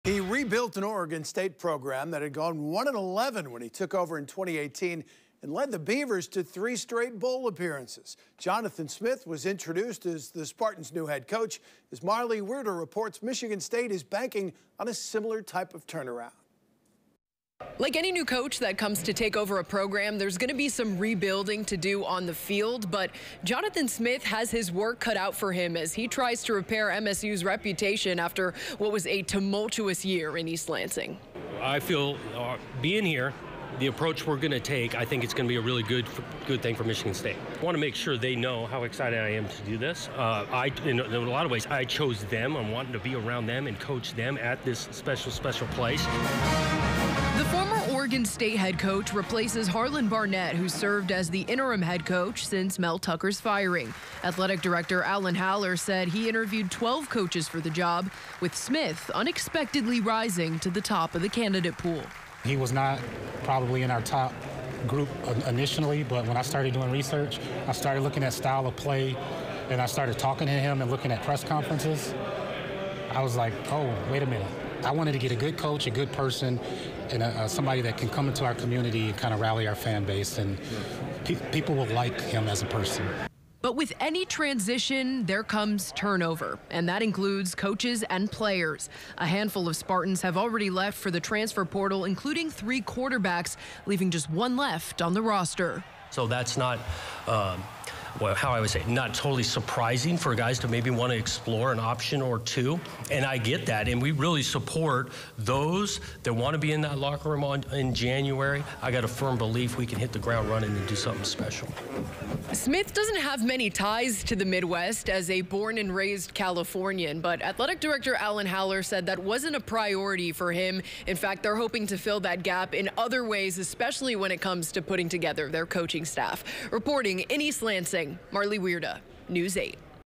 Jonathan Smith takes over at Michigan State
He leaves Oregon State. (Nov. 28, 2023)